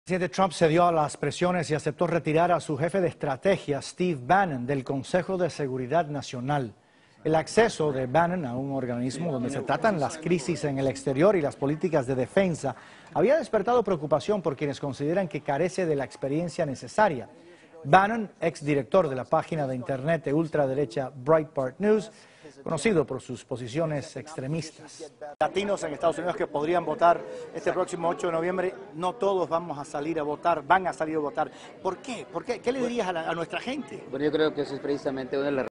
Donald Trump retira a Steve Bannon de Consejo de Seguridad Nacional | Noticiero | Noticias Telemundo
Video oficial de Noticias Telemundo. Conocido por sus posiciones extremistas, este Jefe de Estrategia de la Casa Blanca había sido criticado por carecer de la experiencia necesaria.